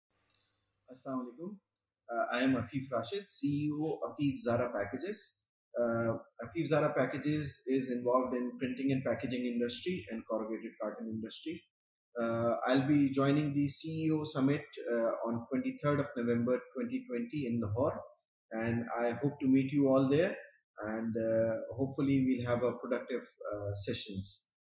CEO Summit Lahore 2022 Shout out by Mr. Afeef Rashid (CEO, Afeef Zara Packages Limited)
Shout out by Mr. Afeef Rashid (CEO, Afeef Zara Packages Limited) for the biggest corporate event of the year CEO SUMMIT Asia Lahore and the Launch  of the Best-selling Book "100 Best Performing CEOs and Companies of Pakistan" on Wednesday 23rd November from 09:00 am to 05:30 pm at Pearl Continental Hotel, Lahore.

The theme for this year is "Turnaround Pakistan - Reshaping the Future".

Mark the date in your calendar to meet the business tycoons and high profiles as more than 200+ CEOs, Federal Ministers, Business Leaders, Policy Makers, Entrepreneurs, Women Leaders will be joining us as Distinguished Delegates at CEO Summit Lahore.